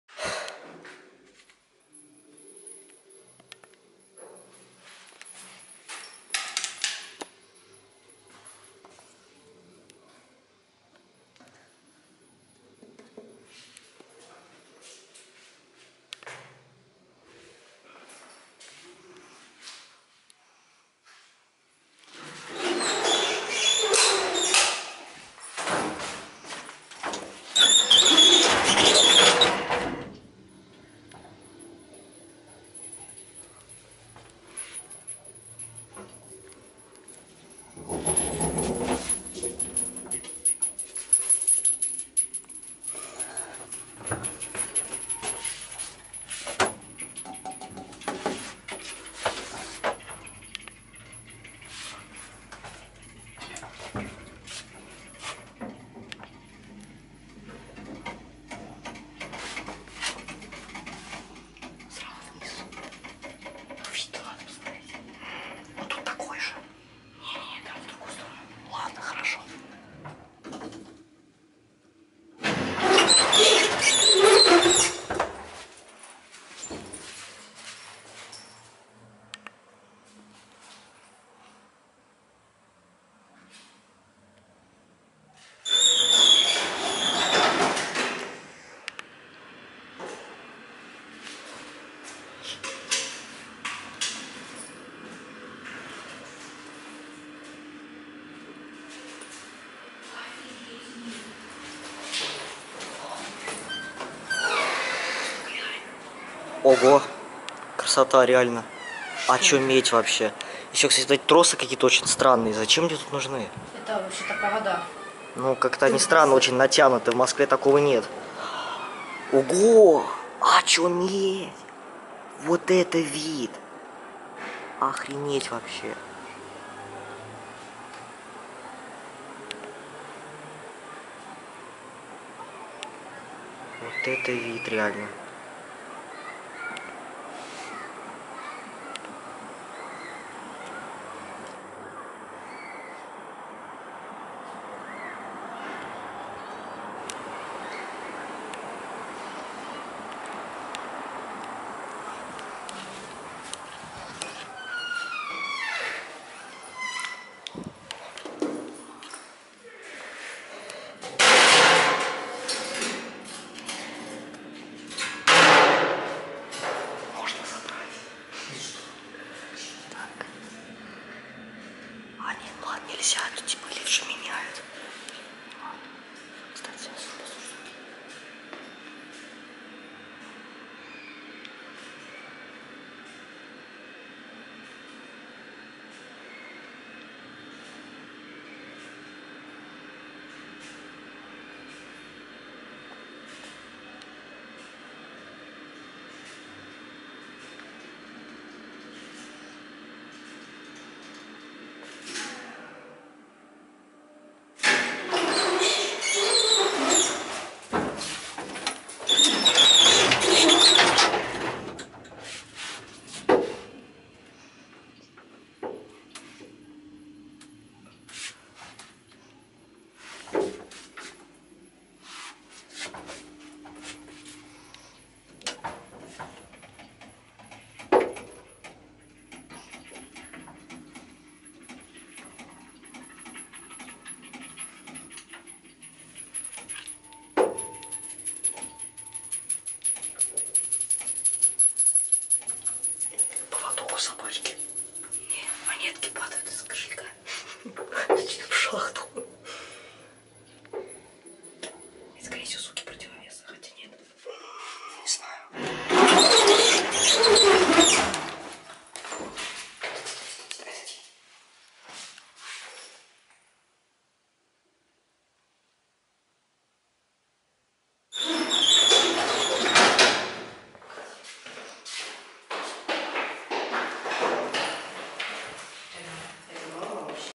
(Раритет) Лифт Строммашина 1973 г. (мод. 2003 г.) @ Адрес скрыт (г. Подольск, МО)
Второй лифт на замене

Снято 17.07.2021 (Sony RX100 M2; w/ IAPPRO)

Консьерж: нет